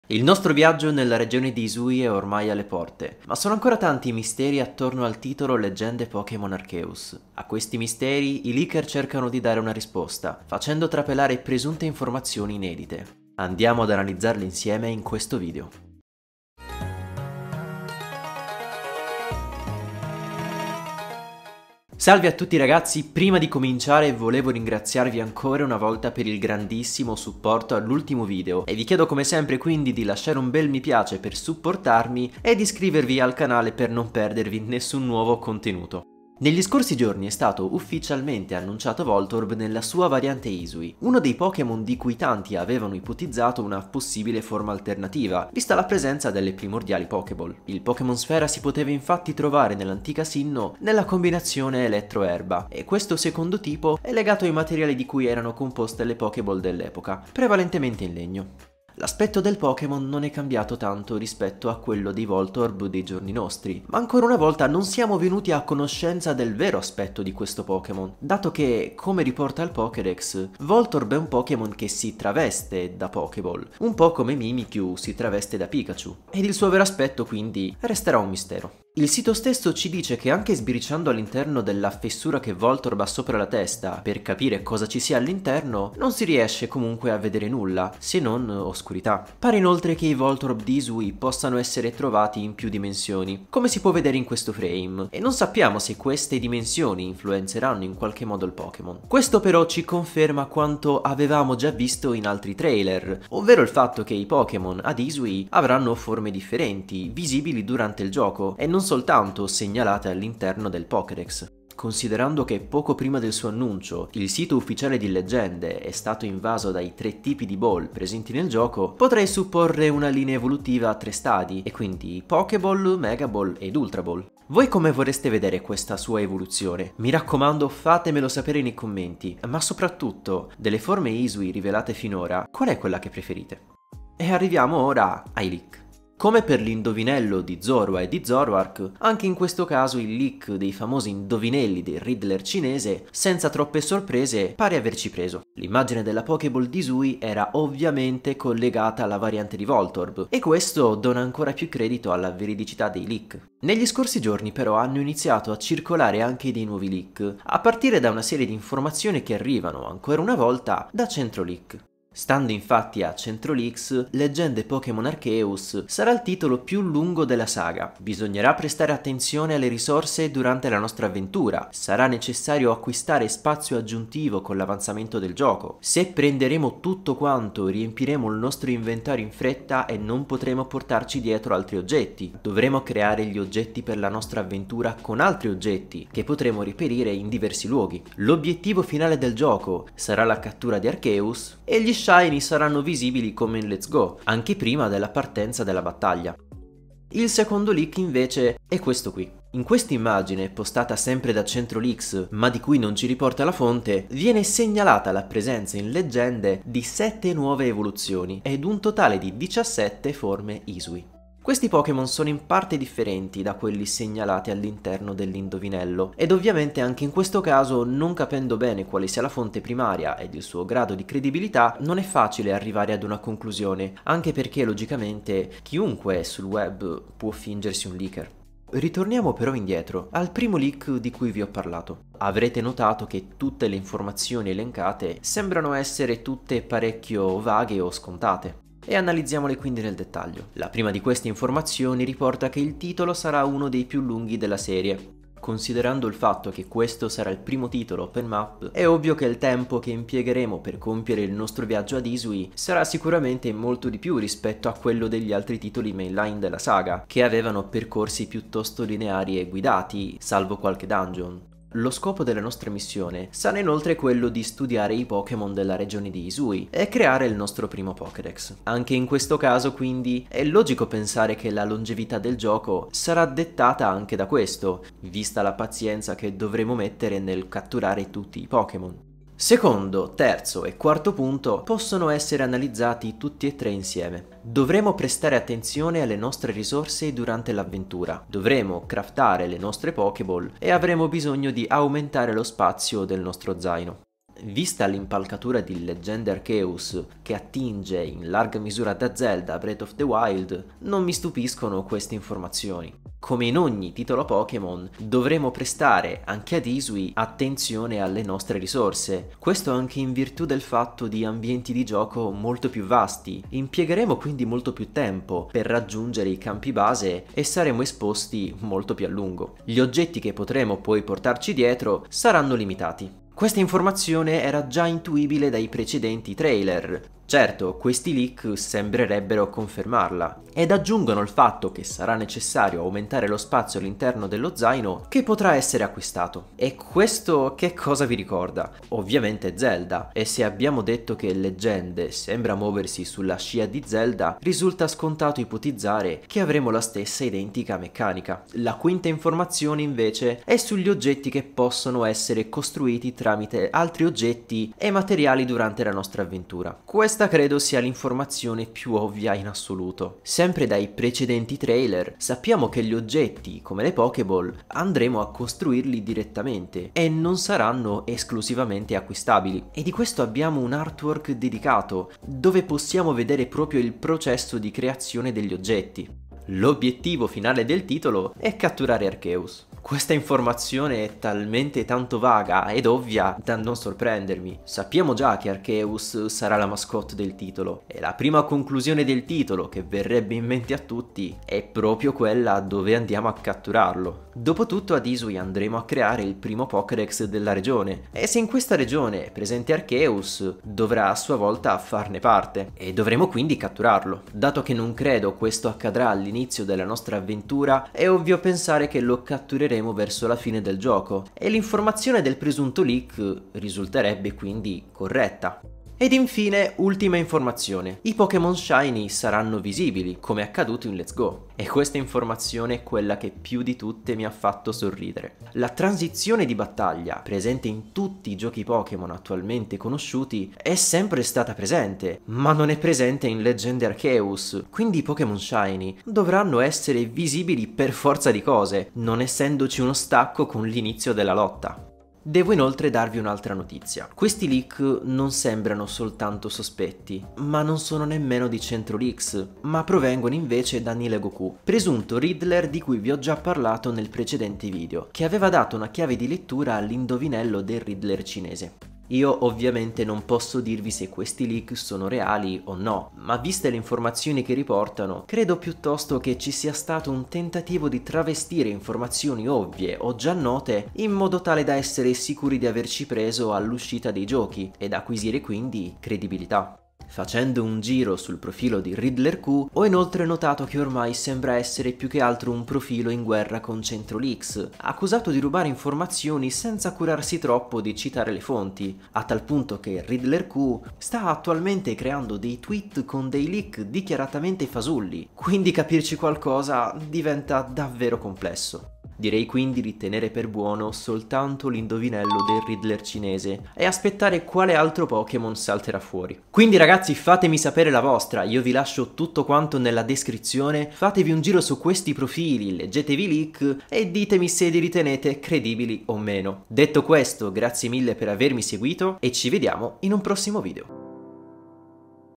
NUOVI LEAK su Pokémon Leggende Arceus: VERI o NO?
Nuovi Leak su Pokémon Leggende Arceus: nuove forme di Hisui, nuove evoluzioni e nuovi elementi di gameplay. La Nuova forma di Voltorb di Hisui li conferma, o ci troviamo davanti ad un falso?

▶Acquista POKÉMON DIAMANTE LUCENTE ad un prezzo SCONTATO!
▶Acquista POKÉMON PERLA SPLENDENTE ad un prezzo SCONTATO!

▶ 10 POKÉMON che DOVREBBERO EVOLVERE in Leggende Pokémon Arceus.